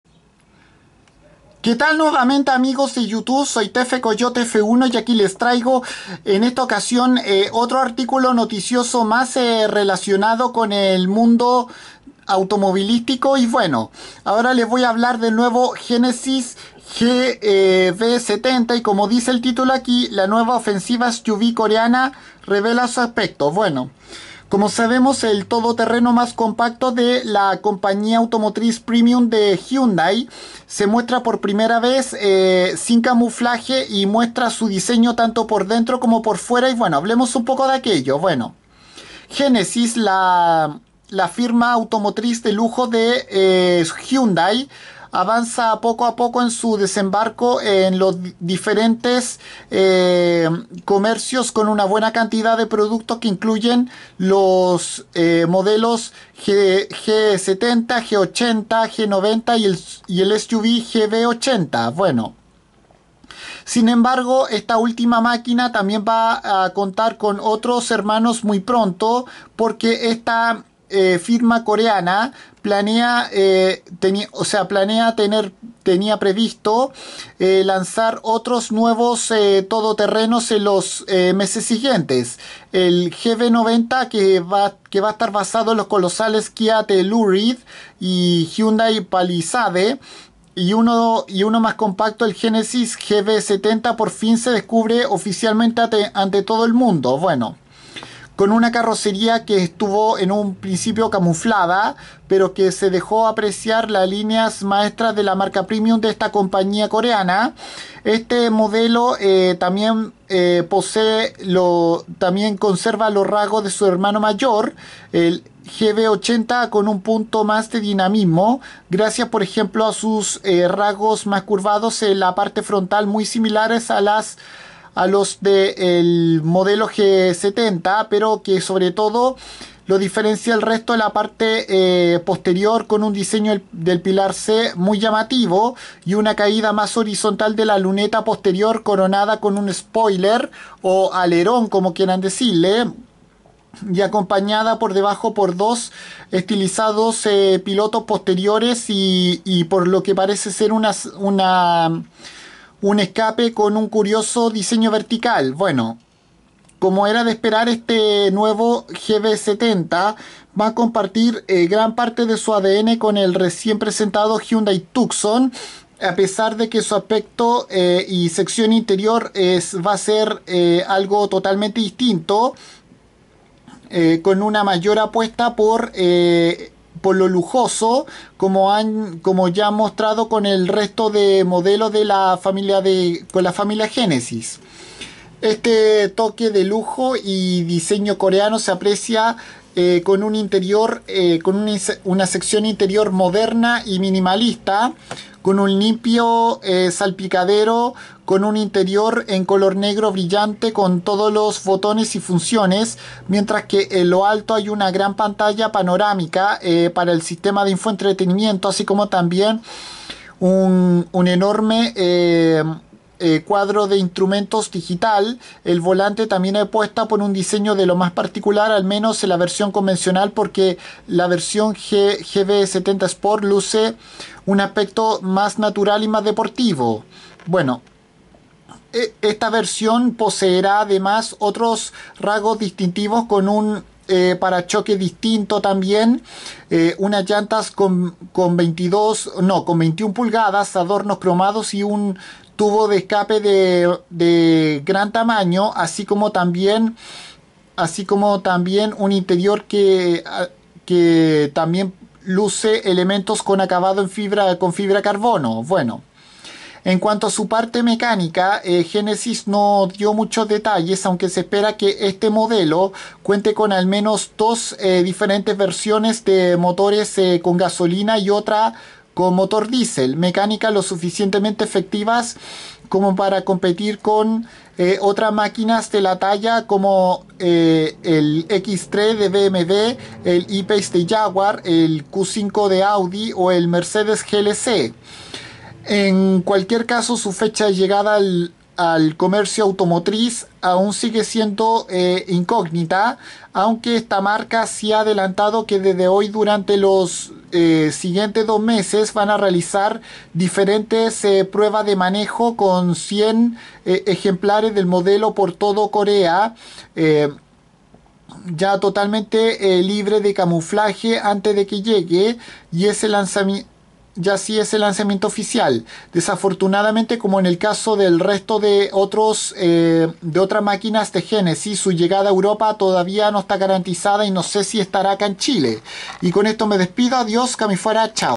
Genesis GV70 información
Aquí les comento sobre el nuevo todoterreno de la nueva marca automotriz premium de la entidad coreana Hyundai que se muestra por primera vez con su carrocería sin camuflaje y también muestra su diseño en su sección tanto interior como exterior